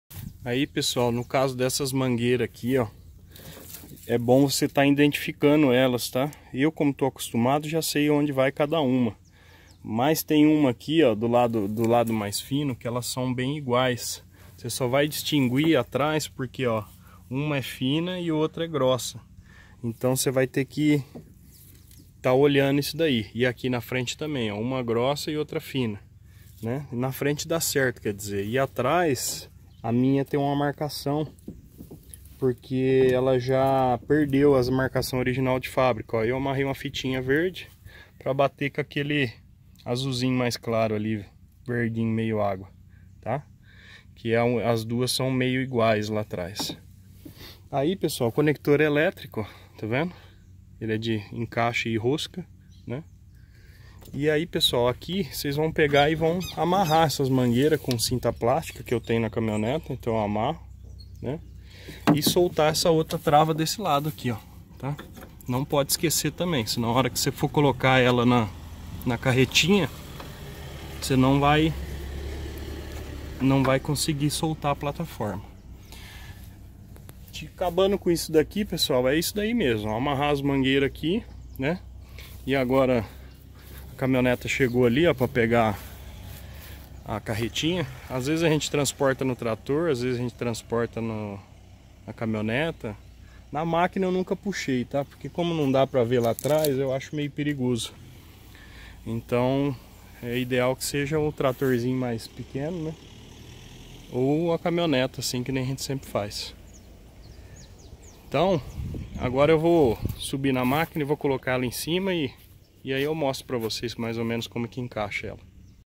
Desengate da plataforma 2 continuação...
Dicas de desengate de plataforma de soja 622f

Sejam bem vindos ao canal MANUAL DO OPERADOR